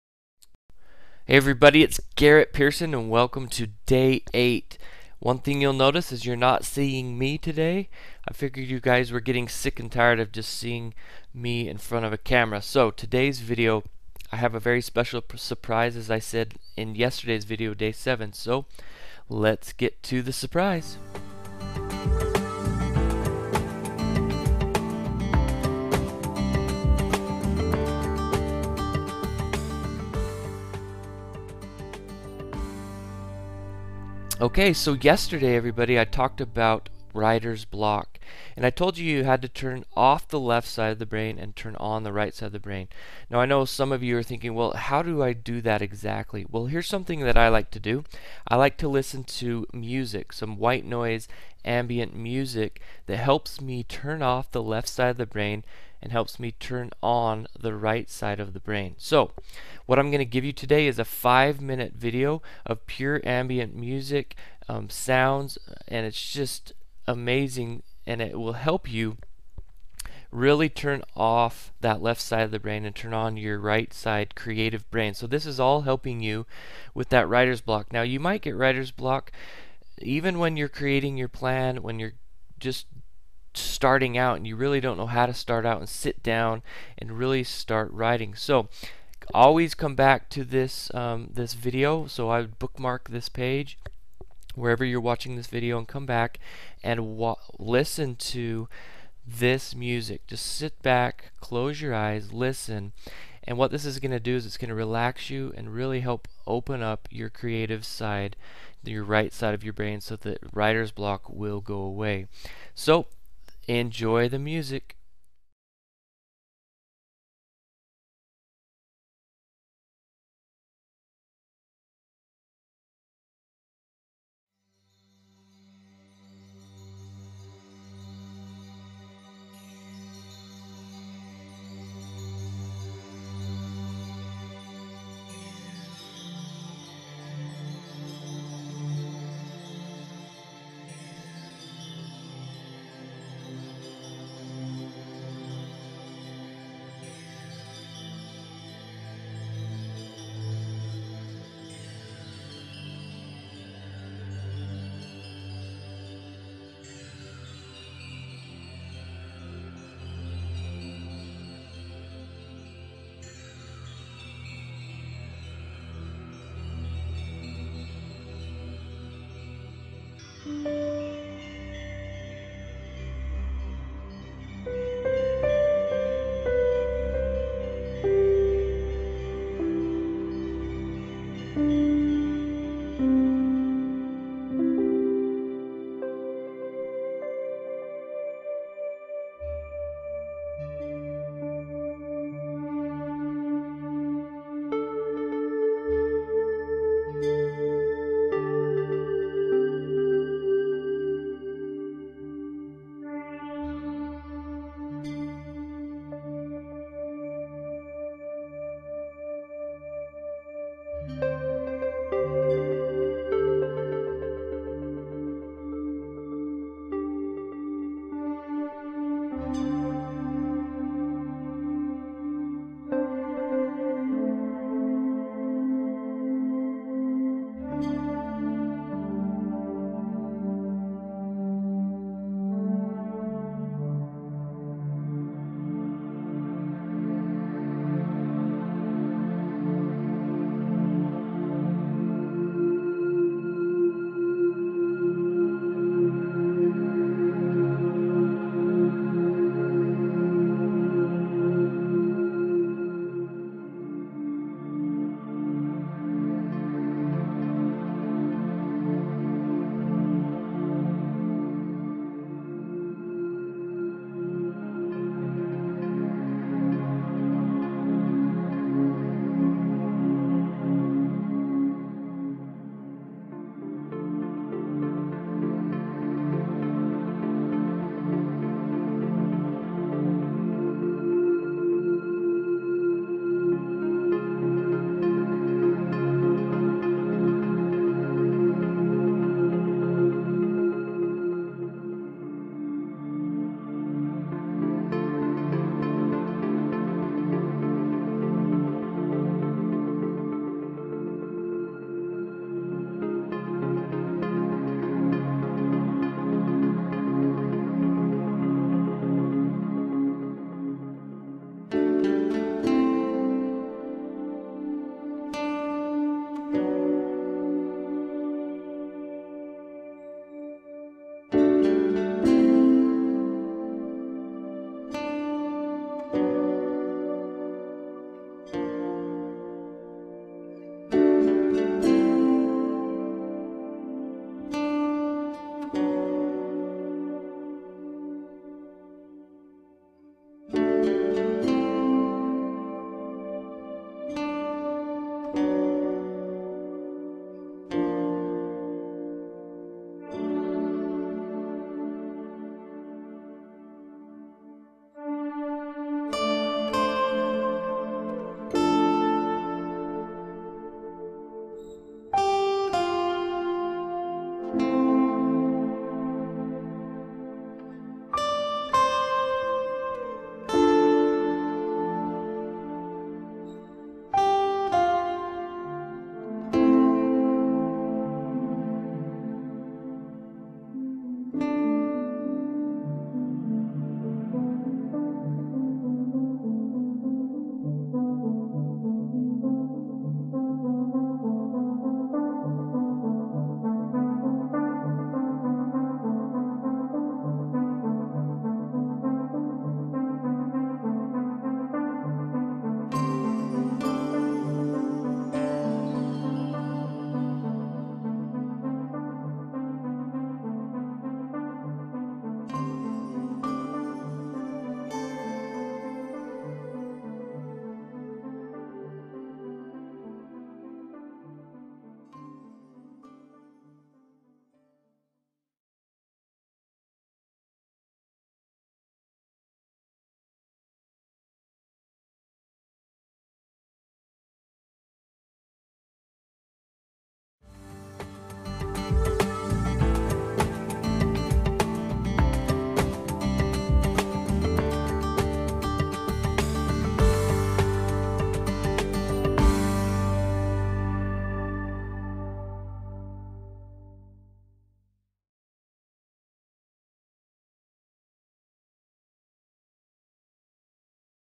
Writers Block Surprise Gift - Day 8
- Day 8 video with surprise gift which is a tool to help overcome writers block, enjoy!

If you are new to this video series on "How To Write A Book ASAP" then start here on Day 1 -